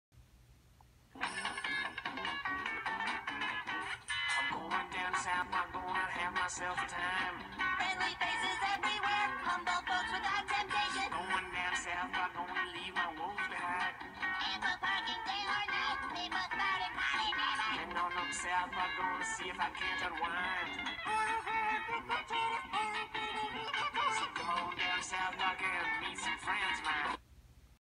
Object Park Preview!
This is a preview of my new show called, Object Park, a Object Show parody of South Park! Coming Soon to YouTube!